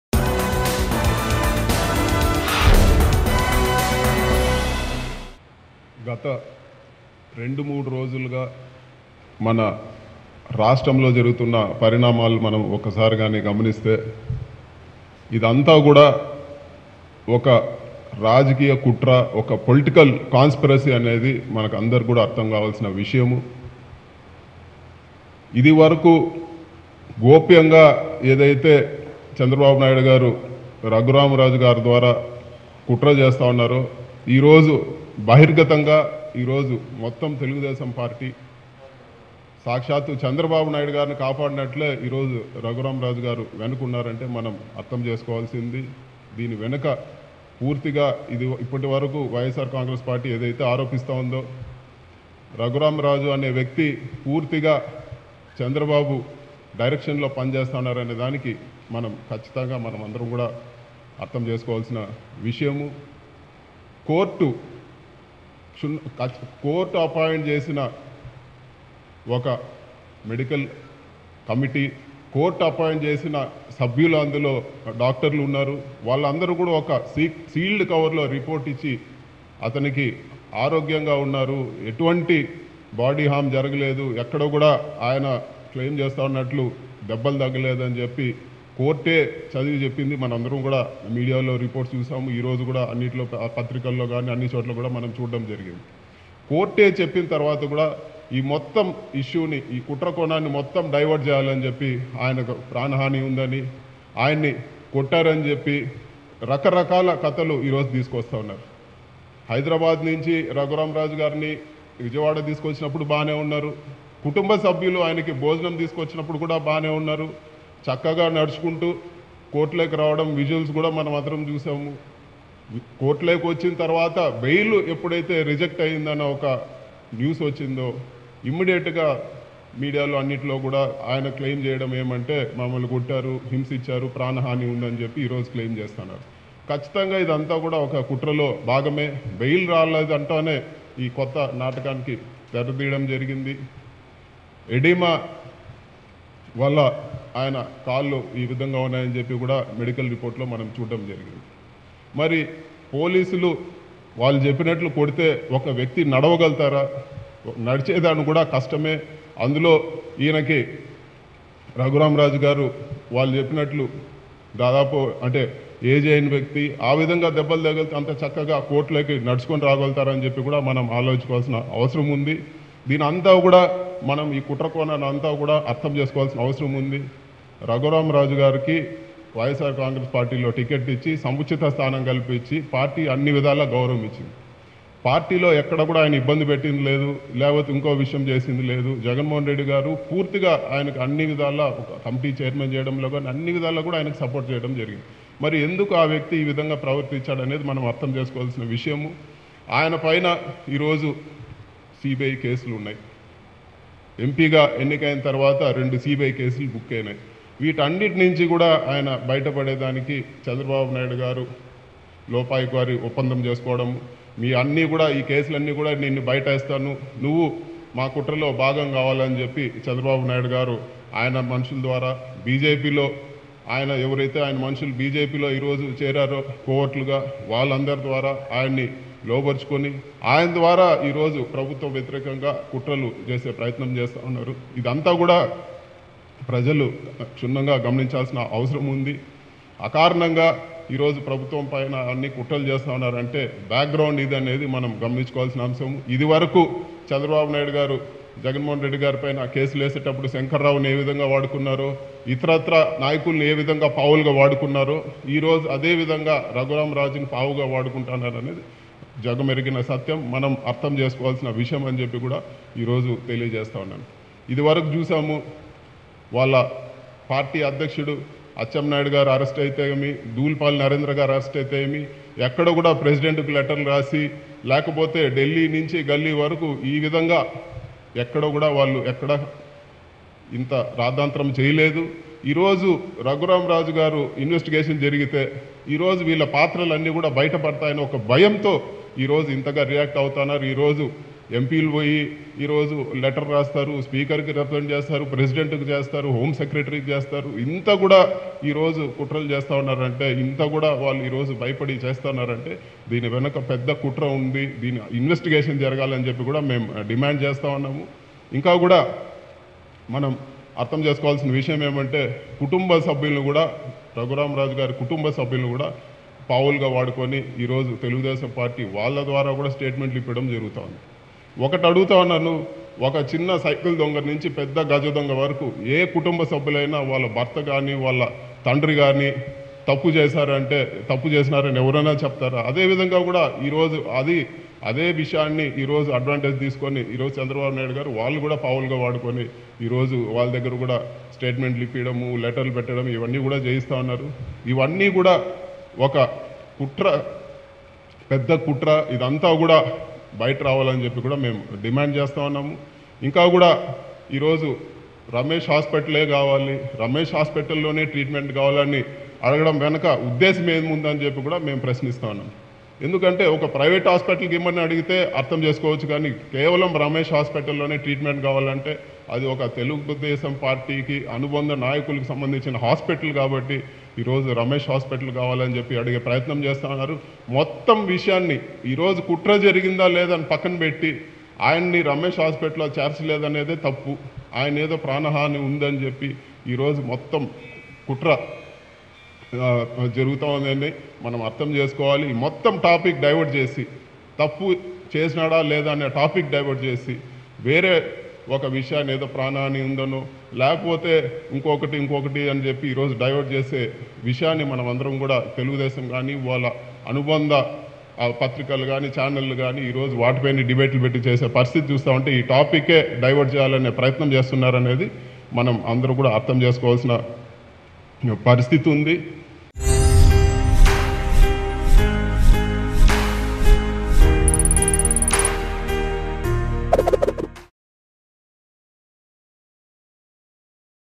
YCP MP Midhun Reddy Reveals Facts About Raghu Rama Krishna Raju | AP Latest News | Mango News
YCP MP Midhun Reddy Reveals Facts About Raghu Rama Krishna Raju.